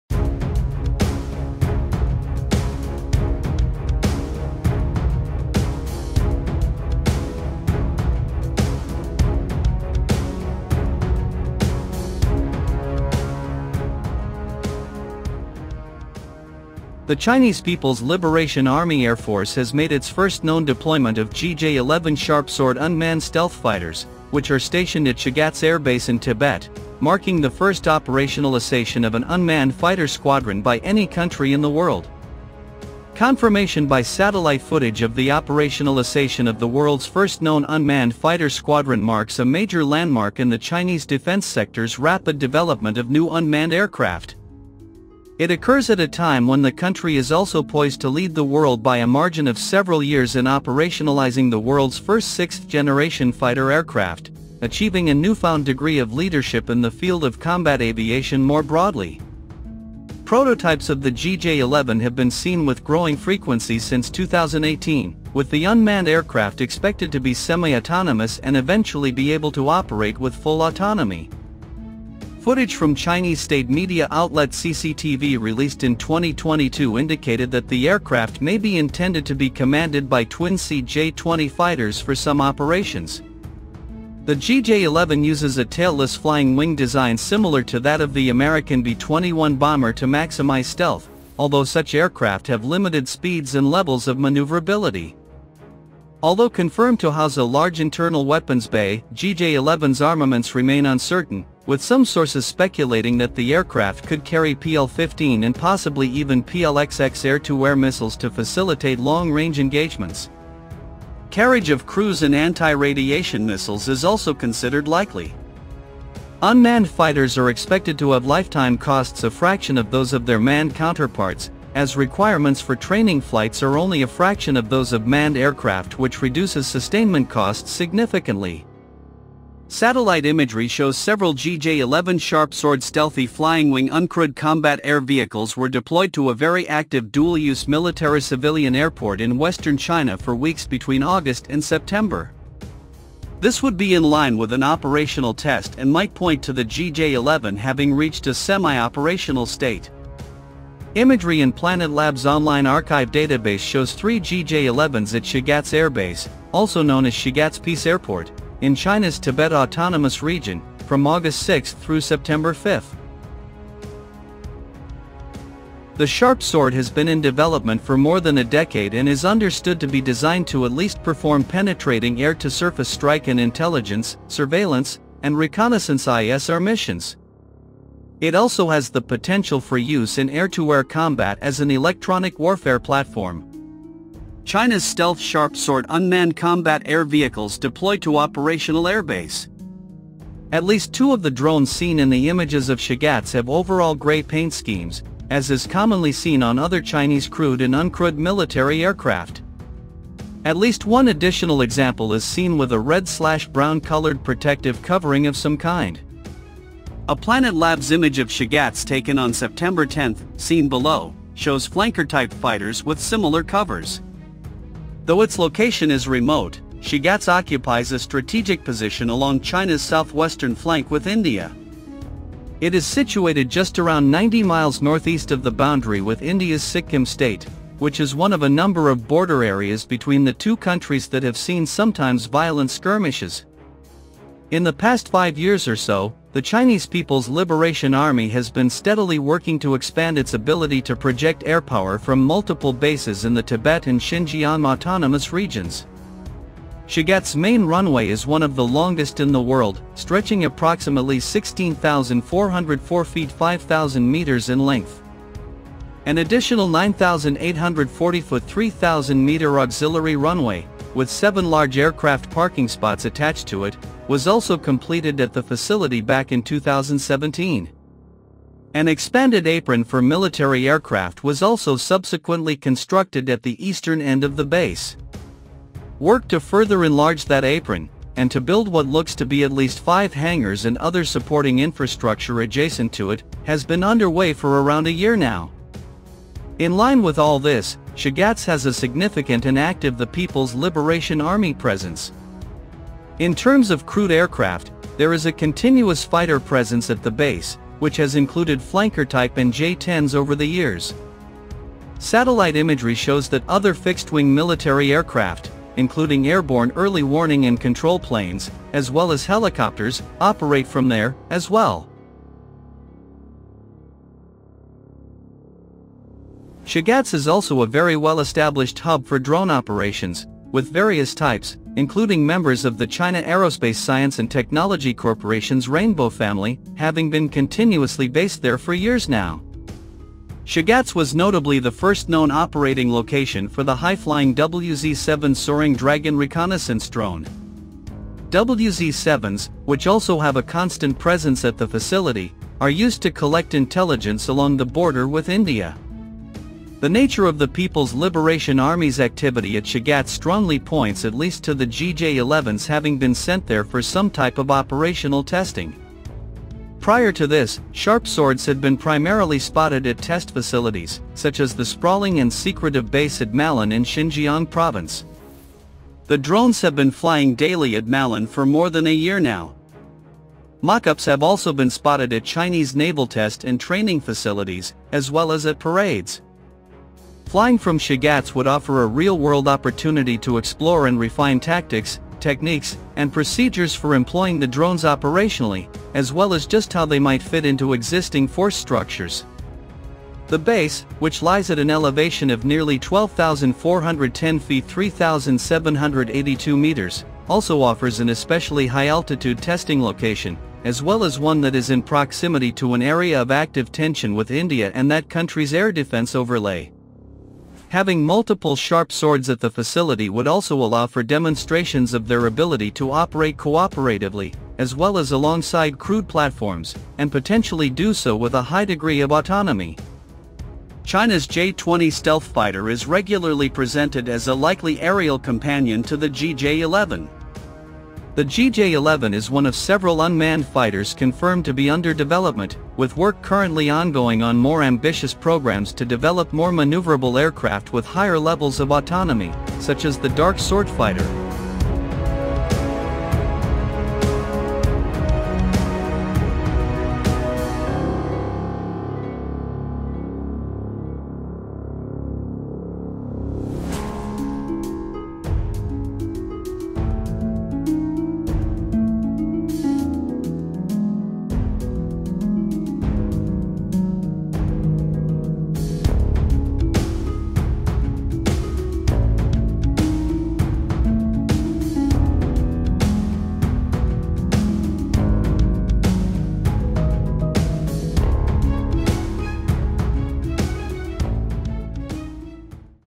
China Deploys World’s First Unmanned Fighter Squadron GJ-11 Stealth Jets Activated in Tibet
The Chinese People’s Liberation Army Air Force has made its first known deployment of GJ-11 Sharp Sword unmanned stealth fighters, which are stationed at Shigatse Air Base in Tibet marking the first operationalisation of an unmanned fighter squadron by any country in the world.